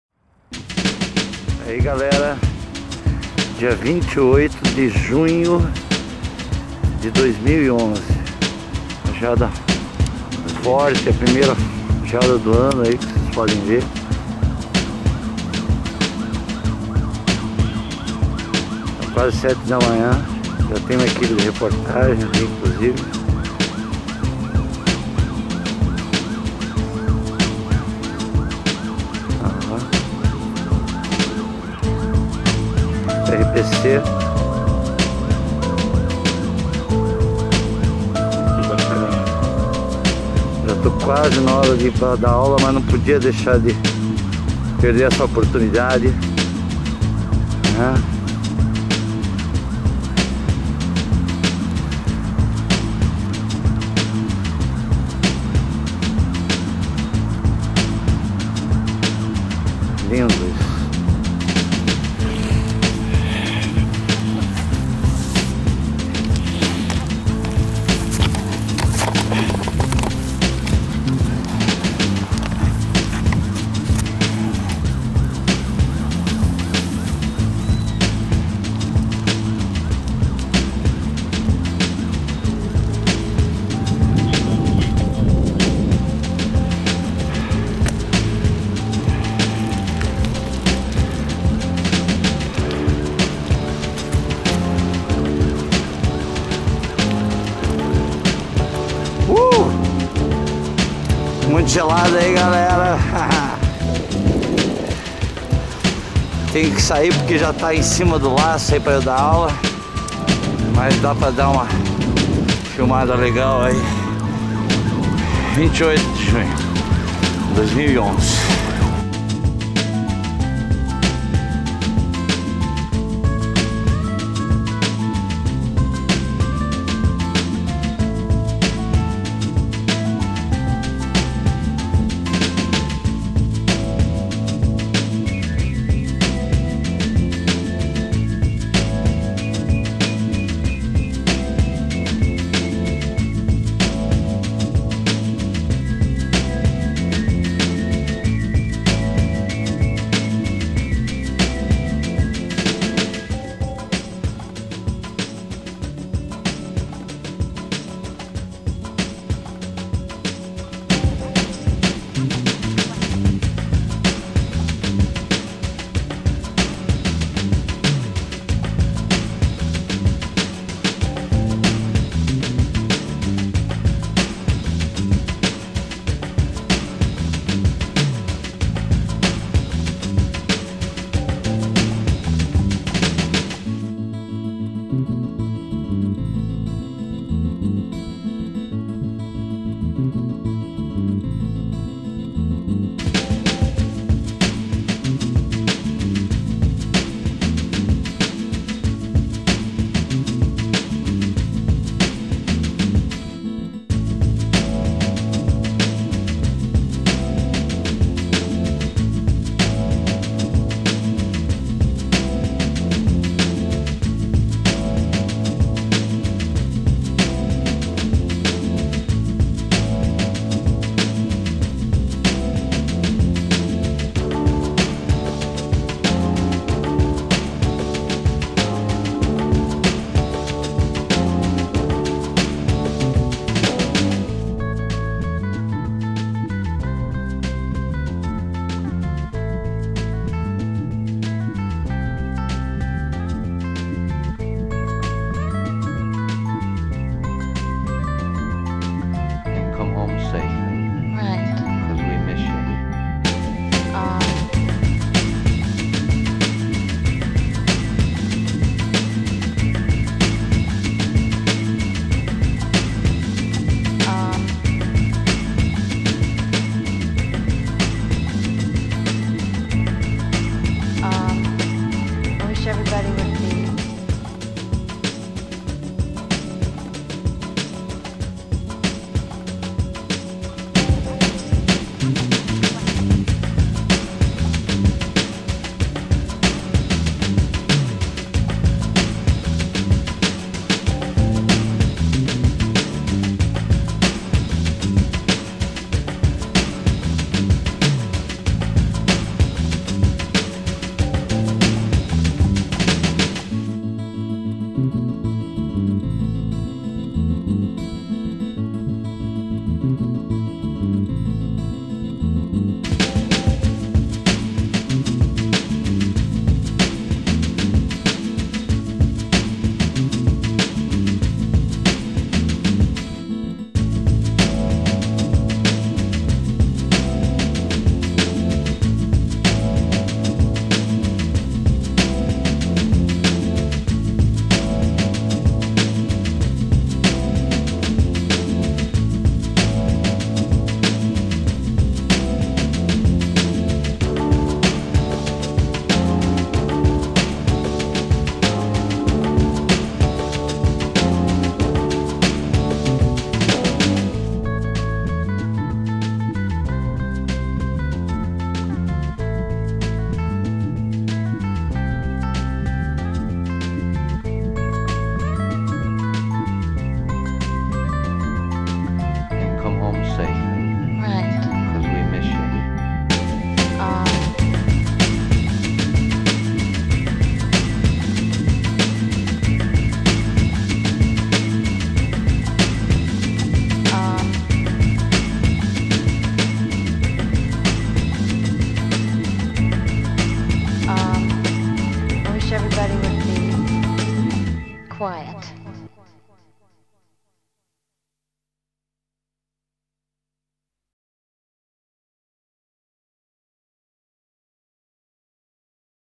GEADA NO PARQUE BARIGÜI
Inverno de 2011, dia 28 de Junho. 7:00hs
Curitiba - PR

imagens e edição:
CELSO RIBAS